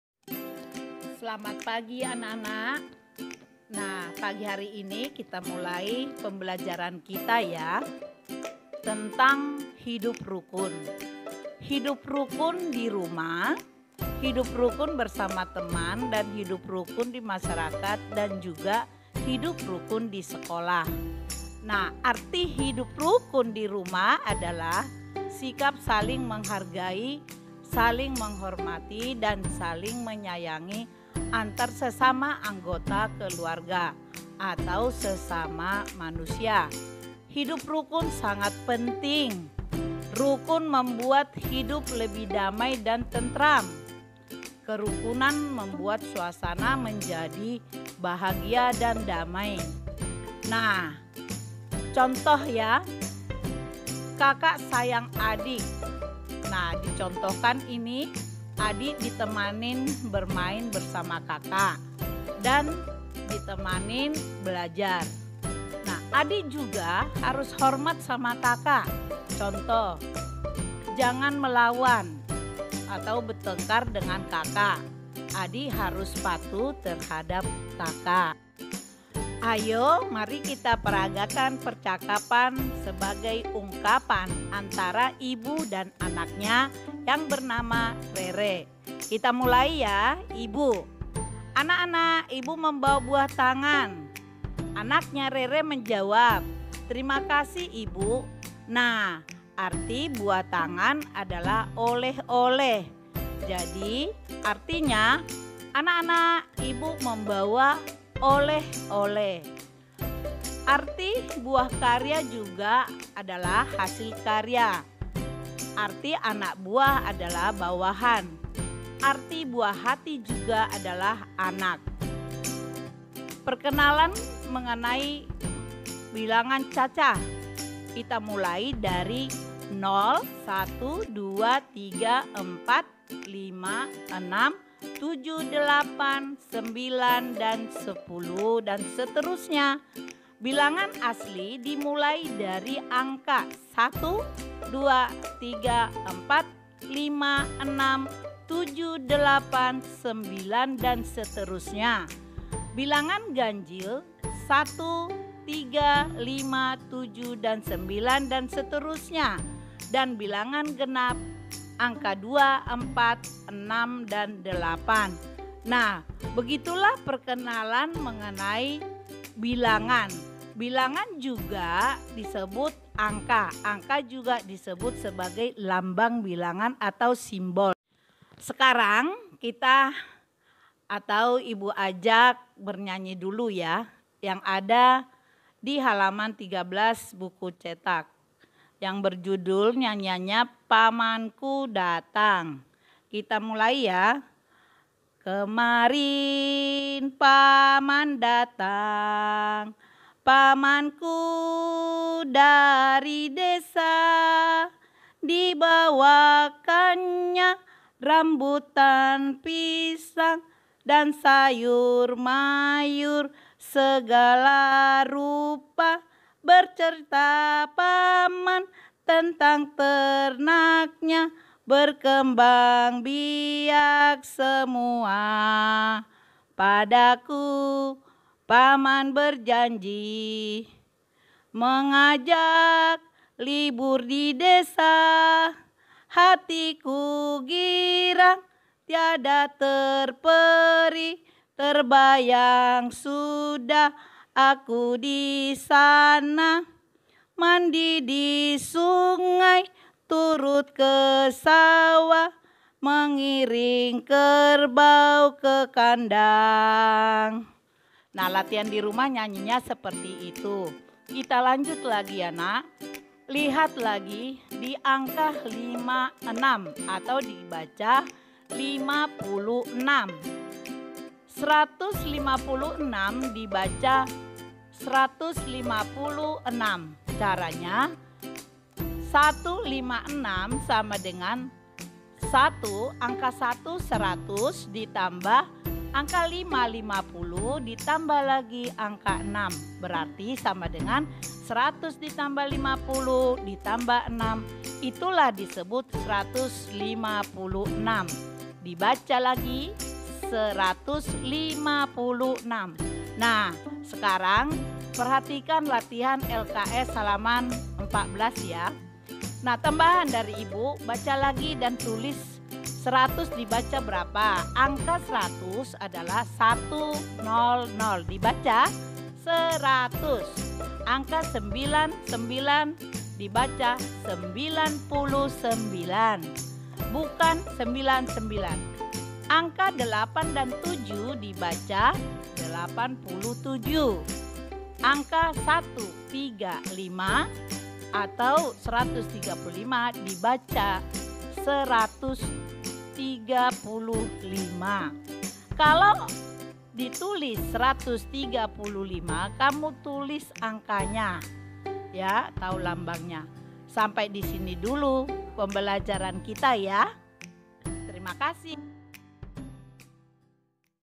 Pembelajaran Hidup Rukun Di Rumah dan Pengenalan Bilangan / Kelas 2B Bu Padama
follow instagram SD Mawar Saron Taman Royal